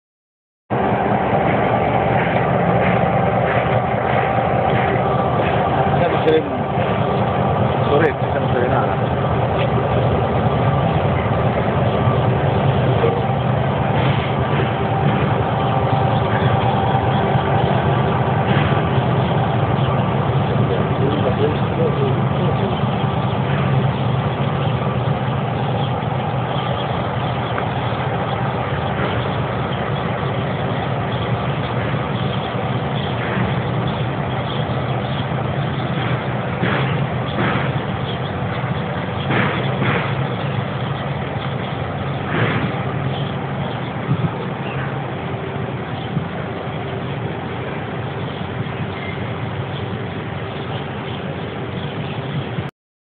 ESTACION CAÑUELAS, MANIOBRAS FERROVIARIAS.
DIFERENTES MANIOBRAS EN ESTACION CAÑUELA.